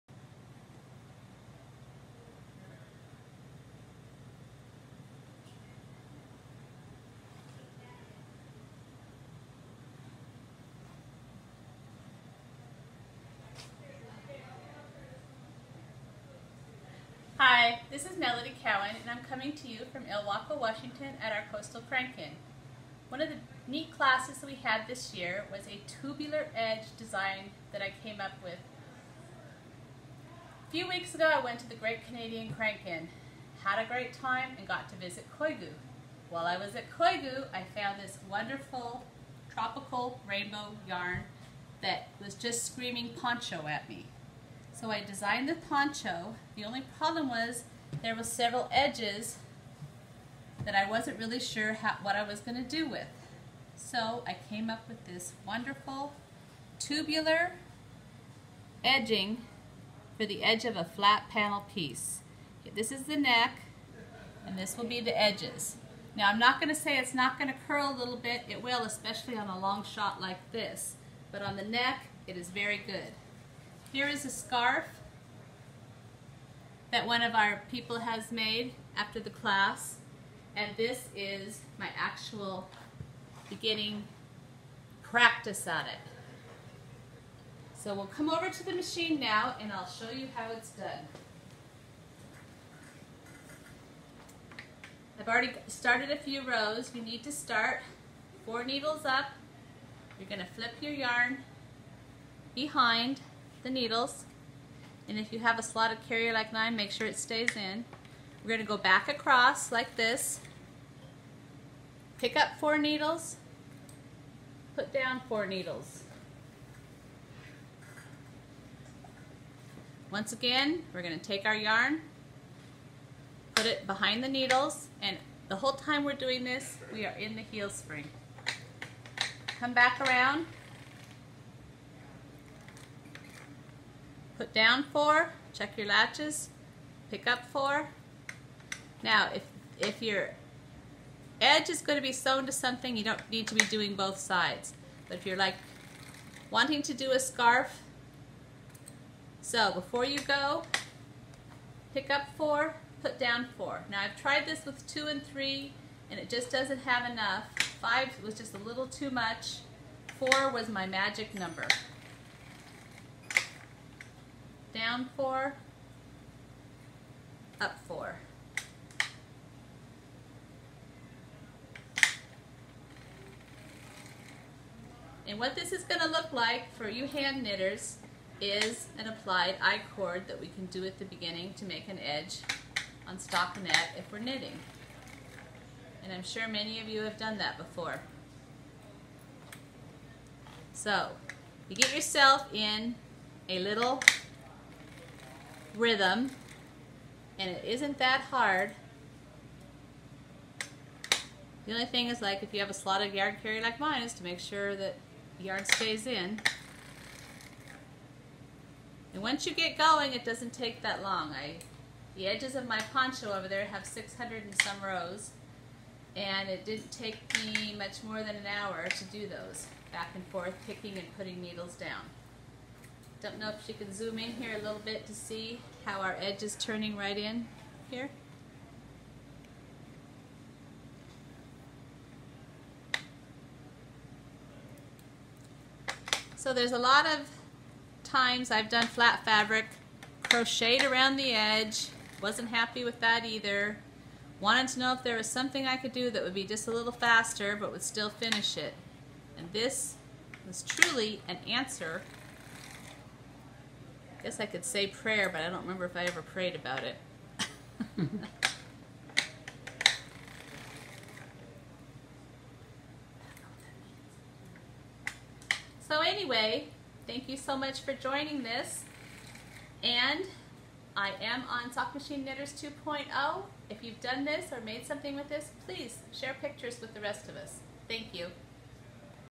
CSM circular sock machine. Melody’s tubular ( I-Cord) edge for Flat Panel knitting
The perfect edging for your for flat panel scarves, ponchos, blankets etc.  Created on circular sock machine as you knit panel.